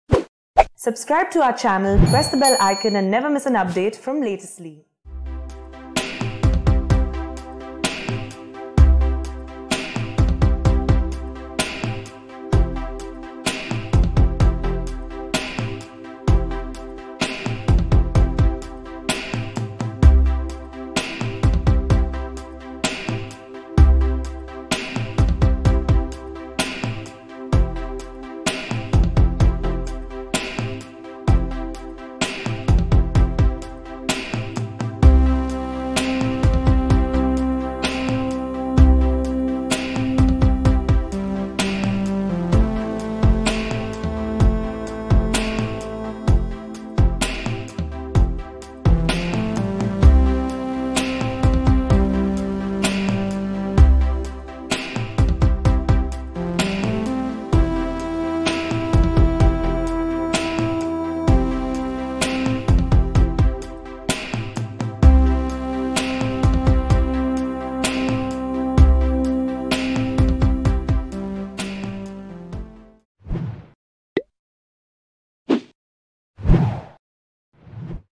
Tomato Price: टोमॅटोच्या वाढत्या किंमतीला आळा घालण्यासाठी केंद्र महाराष्ट्रातून टोमॅटो खरेदी करणार
ग्राहक व्यवहार विभागाने राष्ट्रीय कृषी सहकारी विपणन महासंघ (नाफेड) आणि राष्ट्रीय सहकारी ग्राहक महासंघ (NCCF) यांना आंध्र प्रदेश, कर्नाटक आणि महाराष्ट्रातील बाजारातून त्वरित टोमॅटो खरेदी करण्याचे निर्देश दिले आहेत. गेल्या महिन्याभरात टोमॅटोच्या दराने सर्वोच्च वाढ नोंदवलेल्या प्रमुख विक्री केंद्रांमध्ये टोमॅटो वितरित करता येईल, जाणून घ्या अधिक माहिती